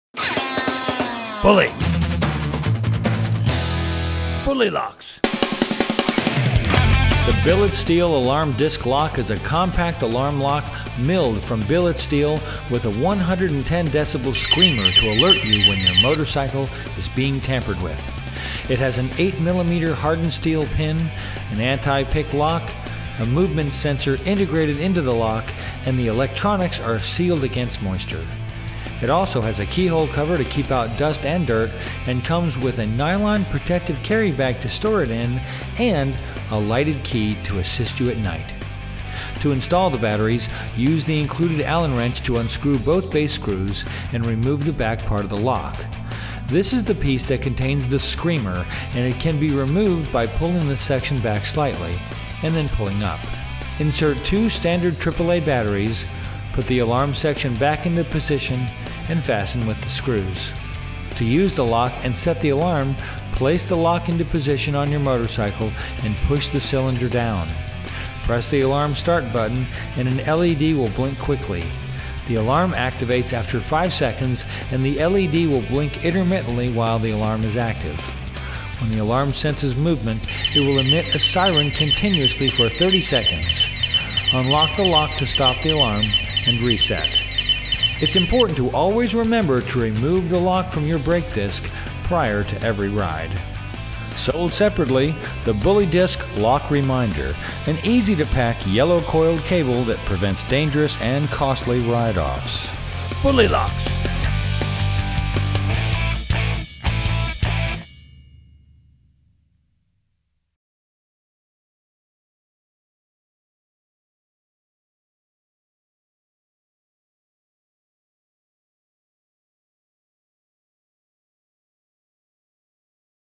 Bully Locks Billet Steel Alarm Disc Lock
• 110db screamer!!! Milled from billet steel
• 8mm hardened steel pin
• Anti-pick lock
• Movement sensor integrated right into the lock
• Electronics sealed against moisture
• Key hole cover protects against dust and dirt
• Lighted key to assist you at night
• Nylon protective carry bag included
• Beautifully plated and polished finish
• Drill and saw resistant
TR Part # 13-2243